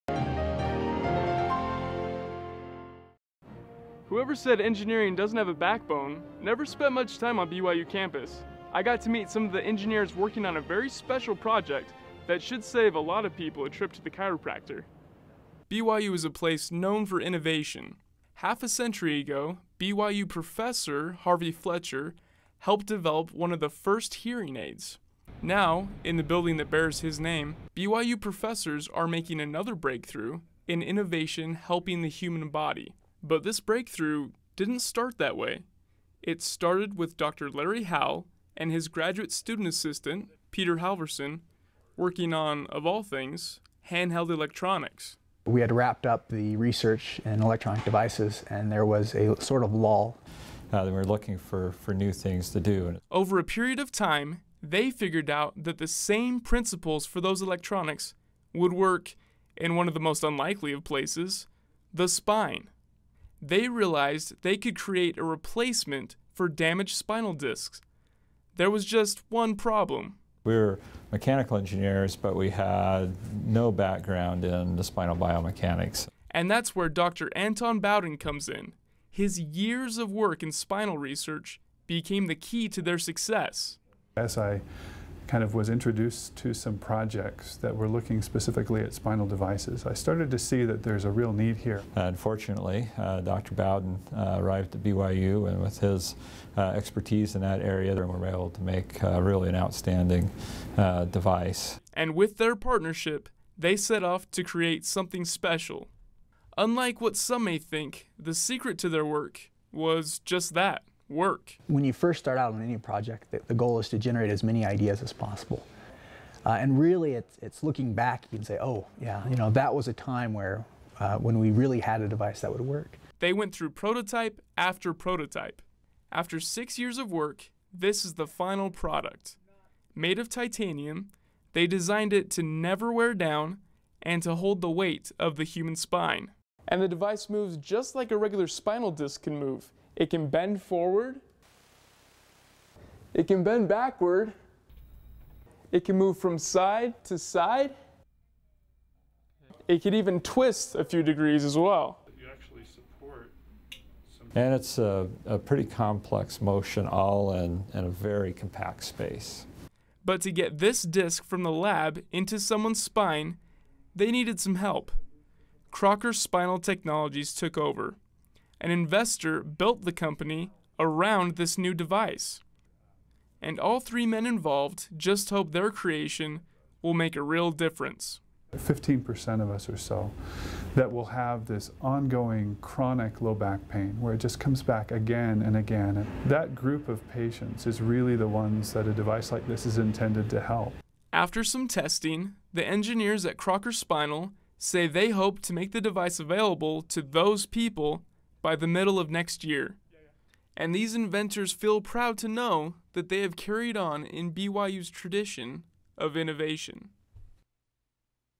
BYU engineers hope to help back pain
Reporter Jeff Merrill shows us an engineering project that should save some people a trip to the chiropractor. See full episode